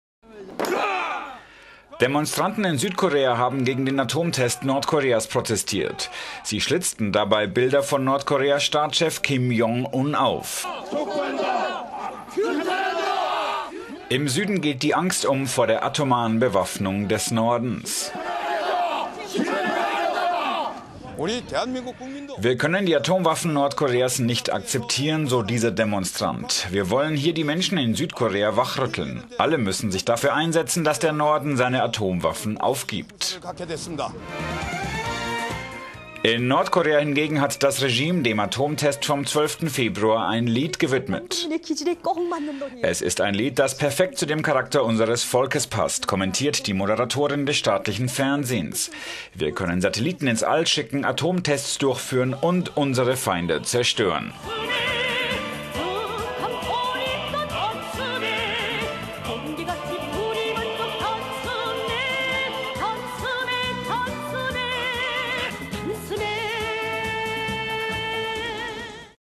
Angst in Seoul vor Nordkoreas Atompotential
Demonstranten in Südkorea haben gegen den dritten Atomtest Nordkoreas protestiert.

Sie schlitzten dabei Bilder von Nordkoreas Staatschef Kim Jong Un auf oder verbrannten Abbildungen des Regimeführers im Nachbarstaat.

Im Süden geht die Angst um - vor der atomaren Bewaffnung des Nordens.

"Wir können die Atomwaffen Nordkoreas nicht akzeptieren", so ein Demonstrant. "Wir wollen hier die Menschen in Südkorea wachrütteln. Alle müssen sich dafür einsetzen, dass der Norden seine Atomwaffen aufgibt."

In Nordkorea hingegen hat das Regime dem Atomtest vom 12.Februar ein Lied gewidmet.

"Es ist ein Lied, dass perfekt zu dem Charakter unseres Volkes passt", kommentierte die Moderatorin des staatlichen Fernsehens. "Wir können Satelliten ins All schicken, Atomtests durchführen und unsere Feinde zerstören."